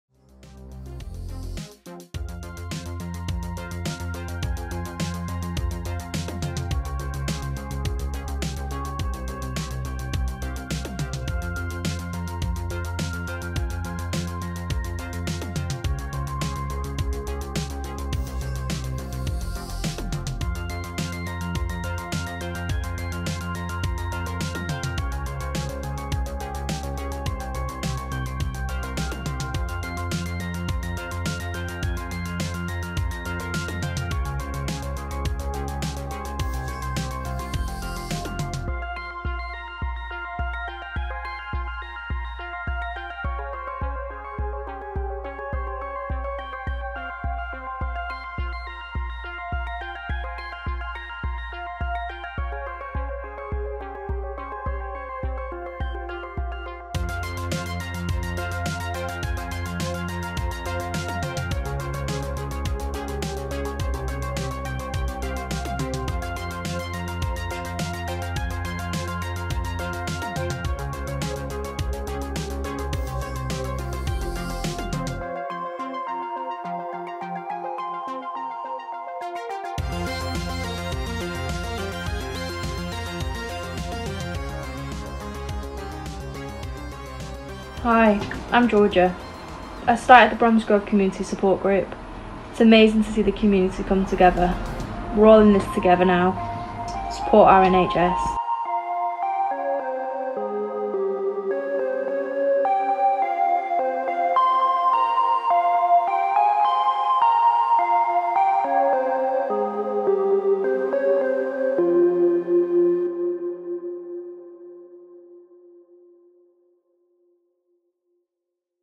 Bromsgrove Community Support Group - COVID-19
In Bromsgrove, Georgia started the Facebook group 'Bromsgrove Community Support' to help elderly and vulnerable people with tasks such as shopping, collection of prescriptions, and providing people with a listening ear.